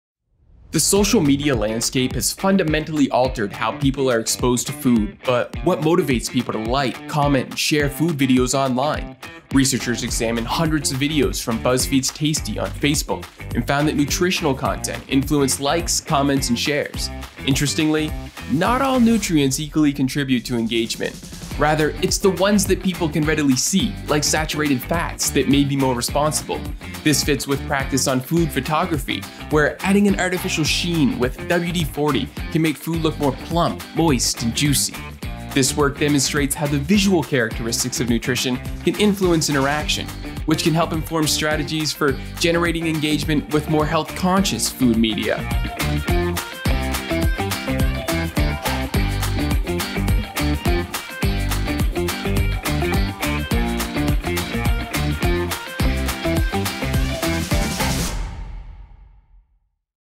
Content Hungry: How the Nutrition of Food Media Influences Social Media Engagement
Journal of Consumer Psychology

Ethan Pancer (Sobey School of Business, Saint Mary's University)
Matthew Philp (Ted Rogers School of Management, Ryerson University)
Maxwell Poole (Sobey School of Business, Saint Mary's University)

Theodore J. Noseworthy (Schulich School of Business, York University)

What motivates people to consume and engage with food media on social networks? We adopt an evolutionary lens to suggest that the valence of people’s affective state varies by the implied caloric density of food media, which has a direct impact on social media engagement. First, we analyze a catalog of Buzzfeed’s Tasty videos based on nutritional content derived from the dish’s ingredients and find that visualizing caloric density (i.e., calories per serving) positively influences likes, comments, and shares on Facebook. We then replicate this phenomenon in an experiment, providing preliminary evidence for the role of affect as an explanatory mechanism. We conclude by isolating the role of affect with a classic misattribution task, which attenuates the elevated engagement resulting from exposure to calorie-dense food media. These findings contribute to the dialogue on the antecedents of social media engagement and offer implications for content developers, advertisers, consumer health advocates, and policy makers.

The authors thank Megan Pollock for her assistance with data collection and gratefully acknowledge Social Sciences and Humanities Research Council of Canada for their financial support. We also thank Priya Raghubir, Tom Kramer, Brent McFerran, and the three reviewers at JCP for their constructive feedback.

This paper was published in Journal of Consumer Psychology and can be accessed